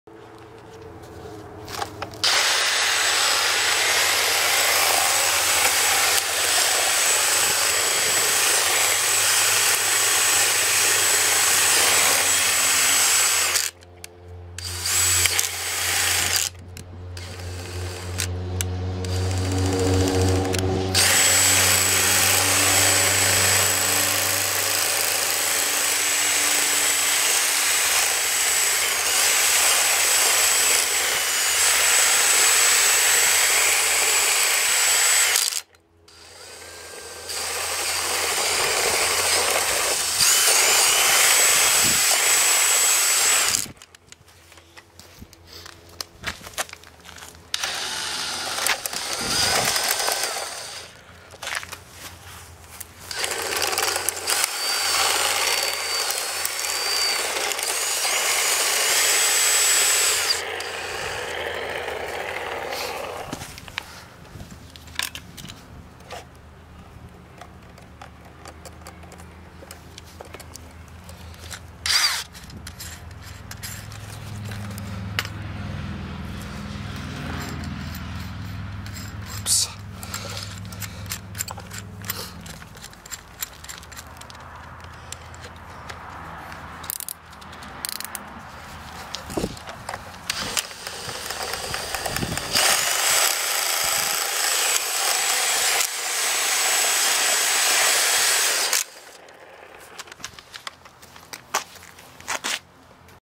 Power Drill Weed Eater
I had an old grass trimmer laying around that would not start no matter what, so I got this idea and wanted to share it.